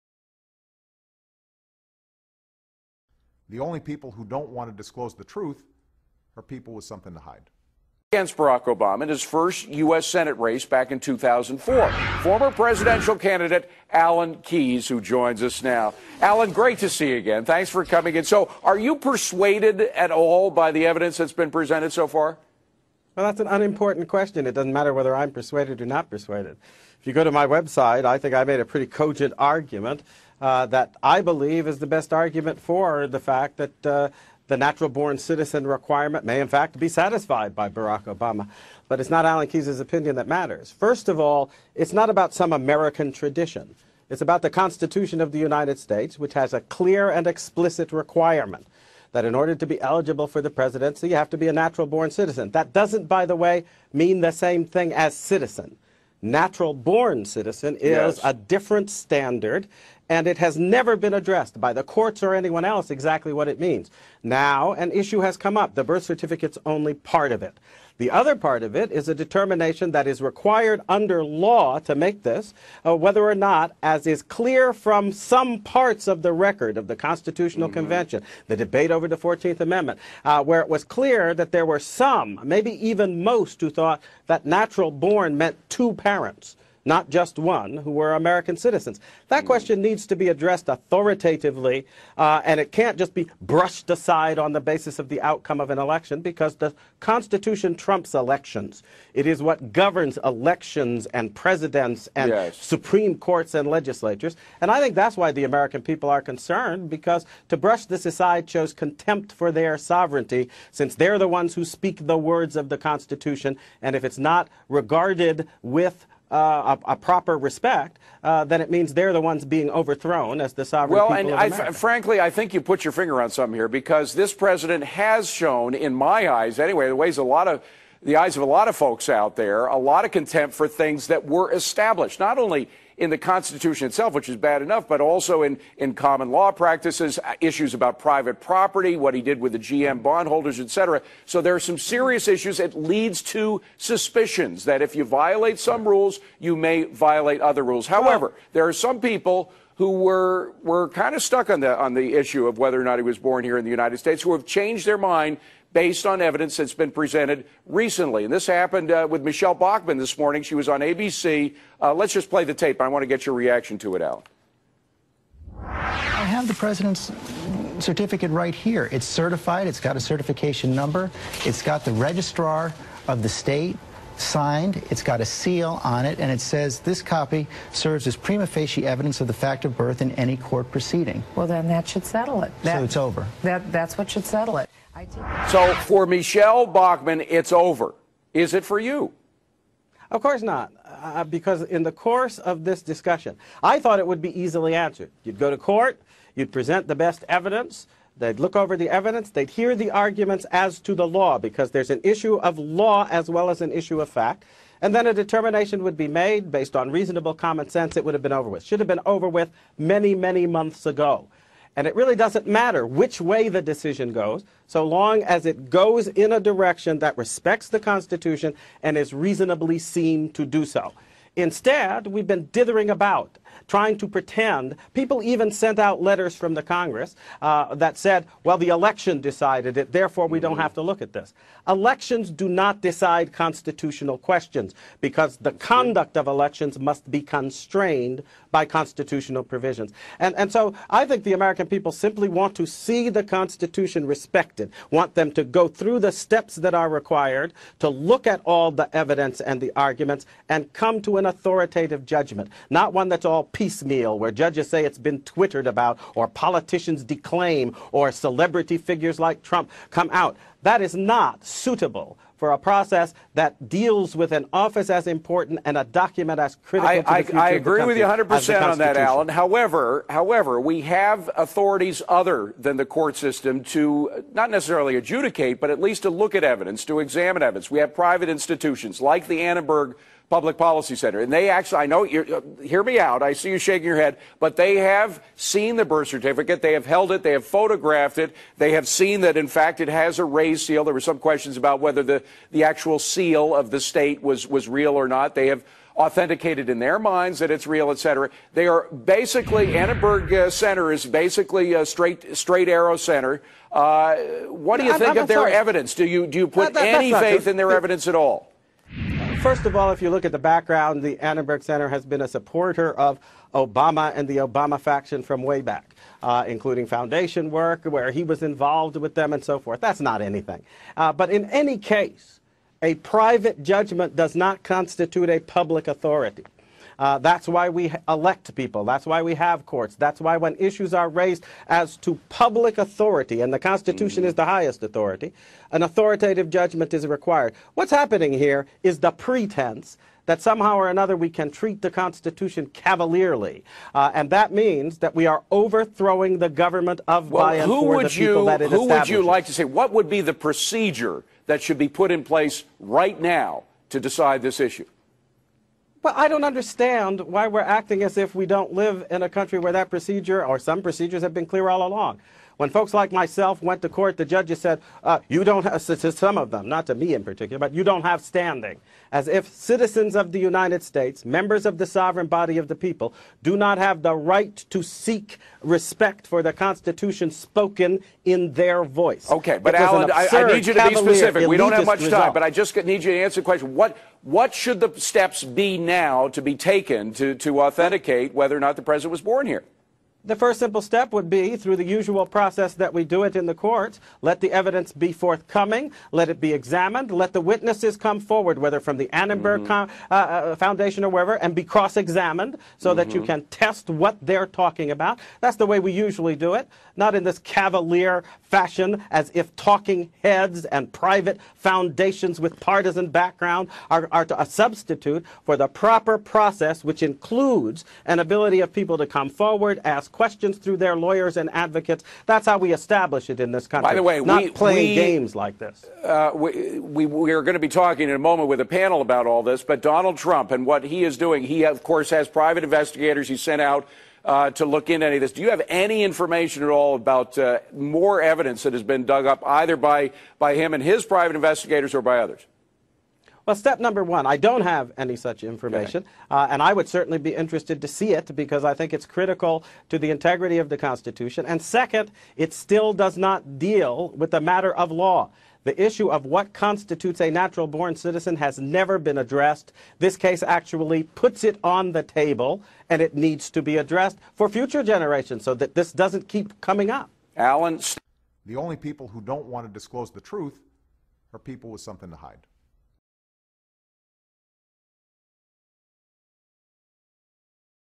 Fox Business Goes Birther ~ Alan Keyes On Citizen Vs. Natural Born Citizen Issue
Bye Bye BoBammie!